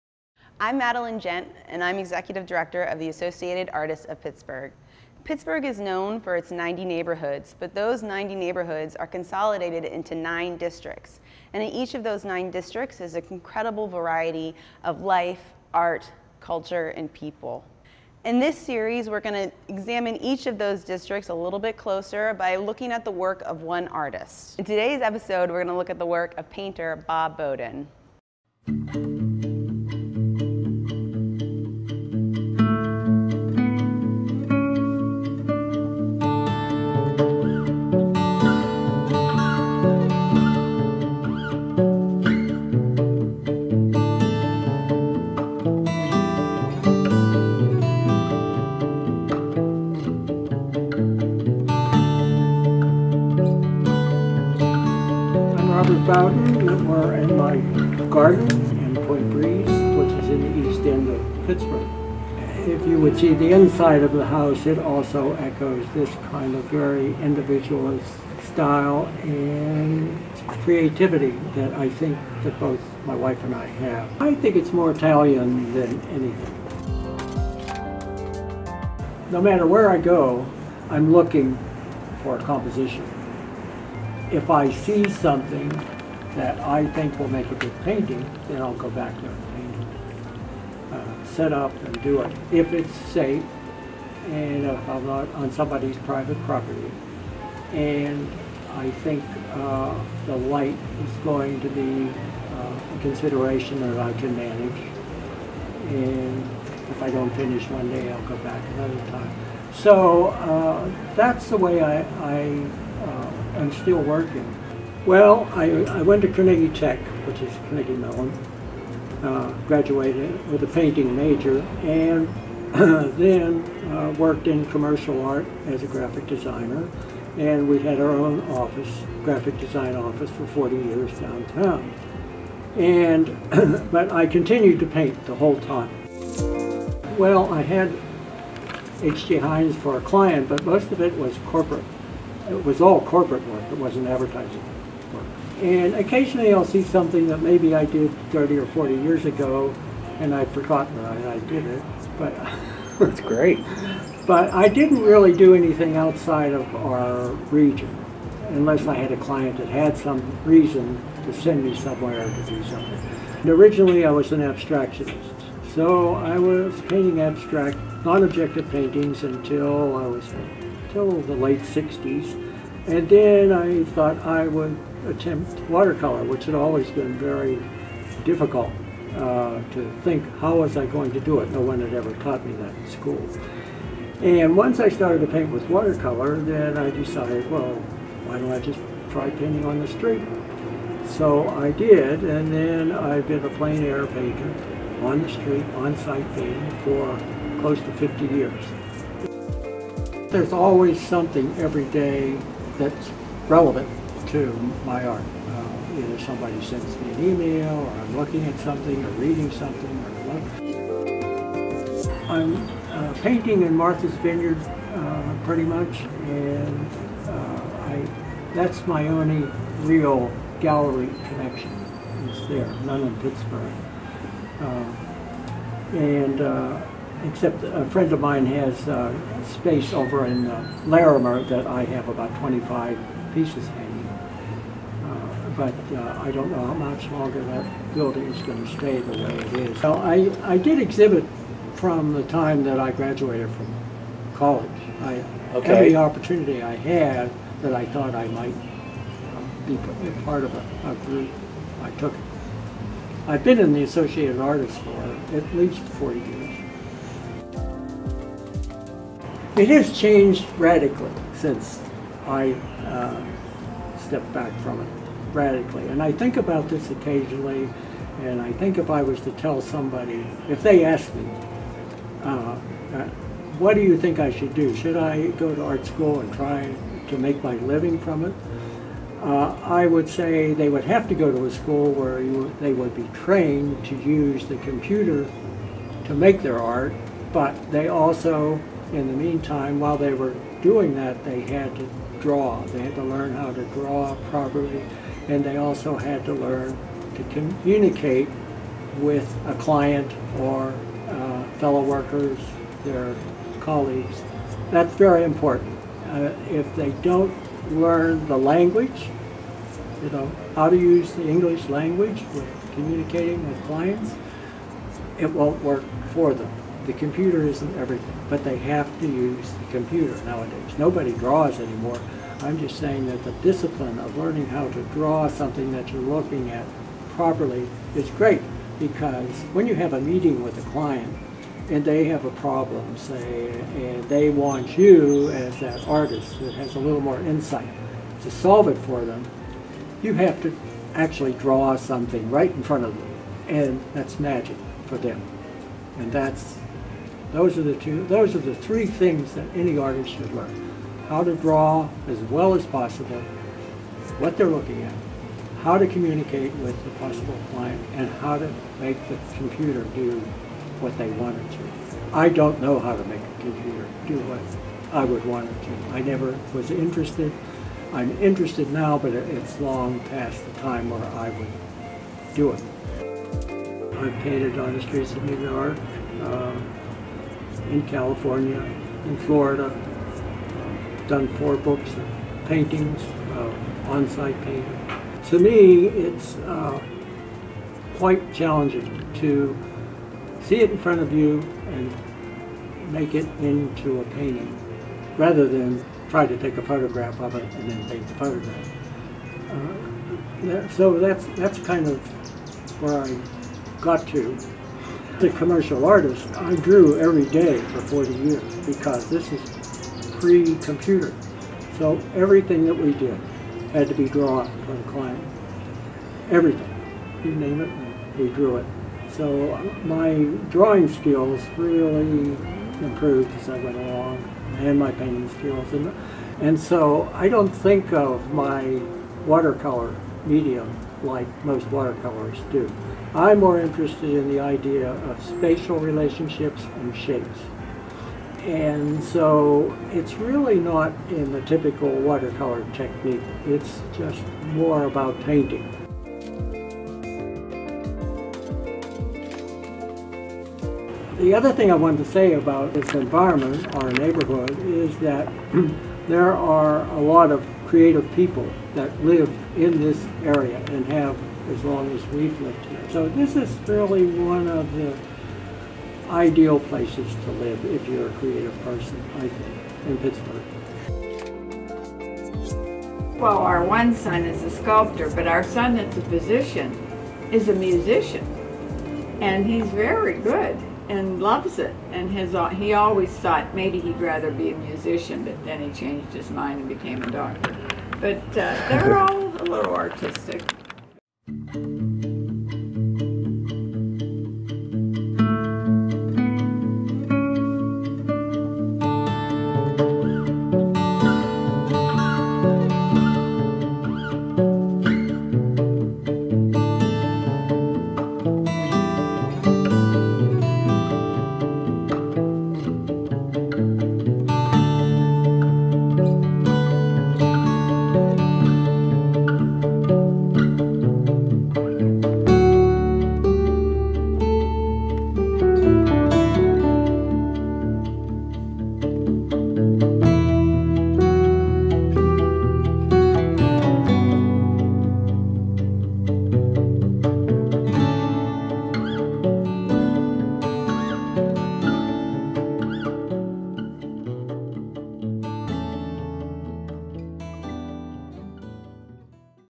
Bob Bowden - 9 Artists 9 Neighborhoods
Robert Bowden has been an Associated Artists of Pittsburgh member for over 50 years. In this video, Robert shares his practice of plein-air painting in his Point Breeze neighborhood and home, as well as  practical advice for young artists in this 9Artists9Neighborhoods video.